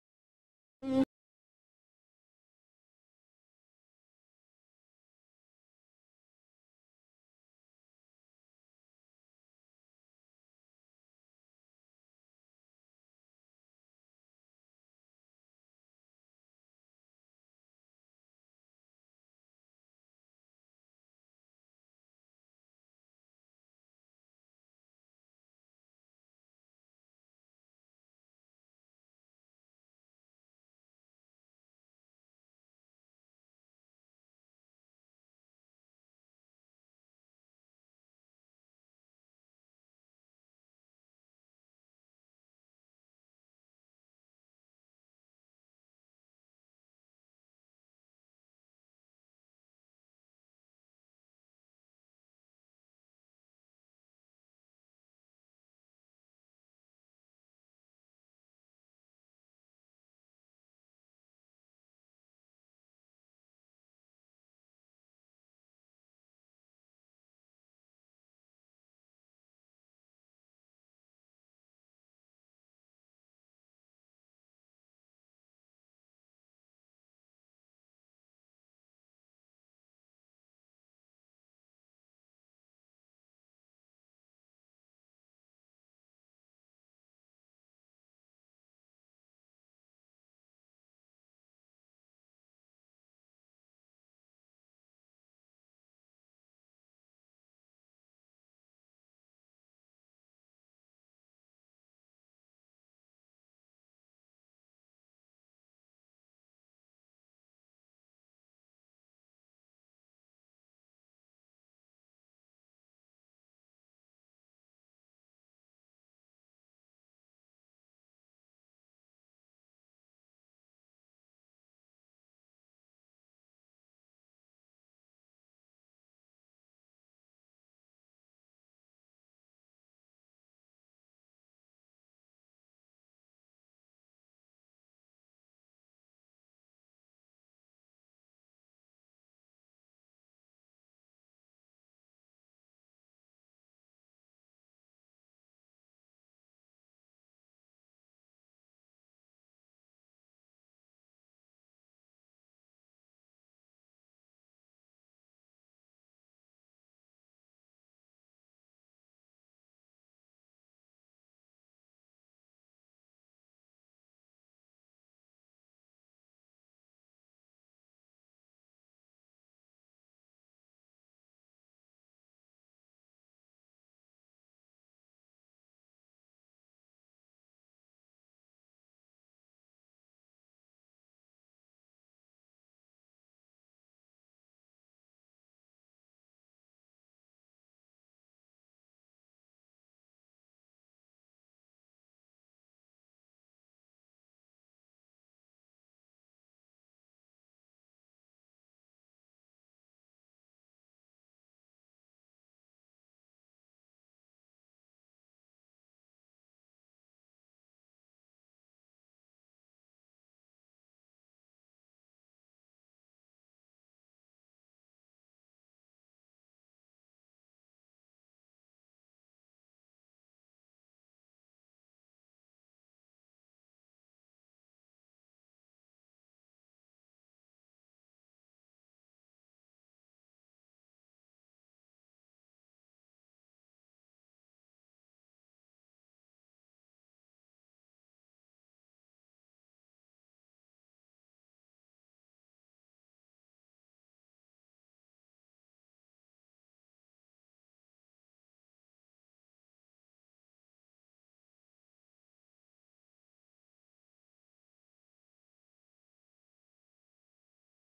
Drawing the Algerian Independence Day as a Girl | Matcha Creates
the background music isn’t mine - credit to the ones who made the music